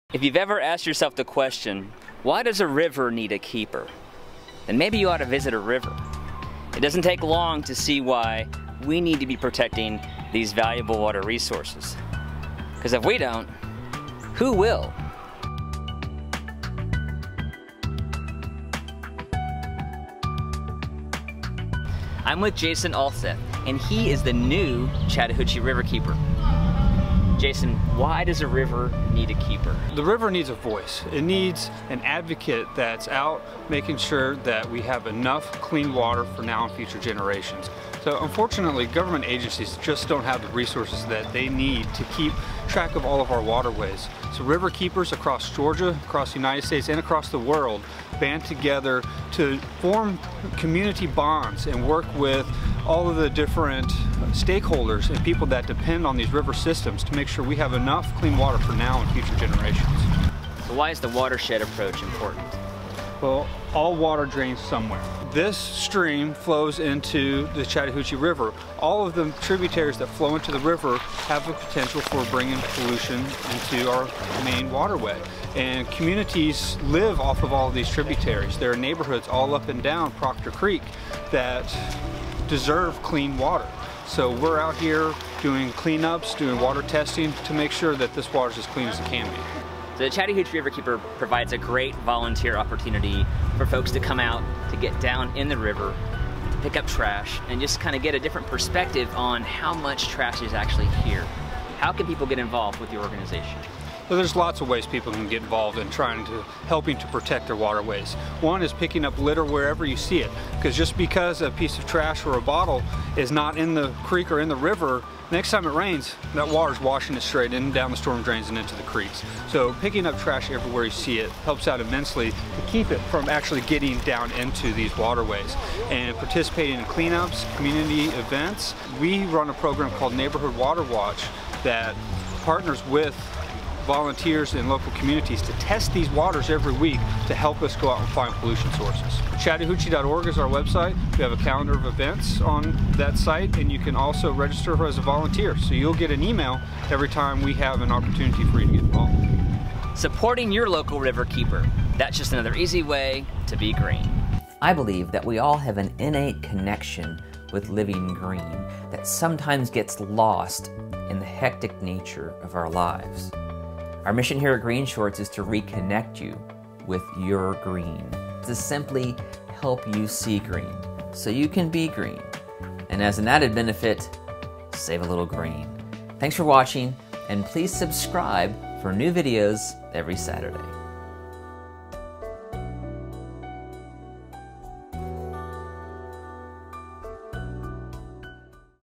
How A Clean Chattahoochee River Means Safer Water For Your Home
Jason Ulseth, the Chattahoochee RiverKeeper, explains why river's need human advocates. They provide important water resources, natural beauty, wildlife habitat and recreation, yet rivers can't speak for themselves...they need us. We must recognize their importance to our communities and make sure they get ongoing protection and help when they need it.

Need a relaxing and fulfilling volunteer opportunity? Find your local RiverKeeper and get your feet wet.